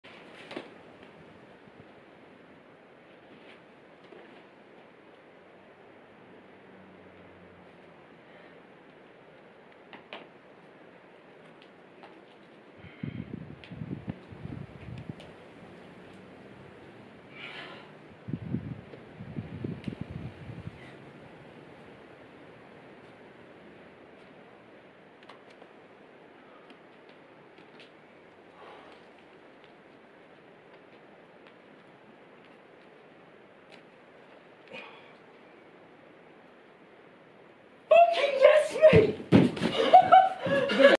Rock Atrocity, 7c. Parisella's Cave
Climber: Daniel Leech
Name: Rock Atrocity
Grade: 7c
Zone: Parisella's Cave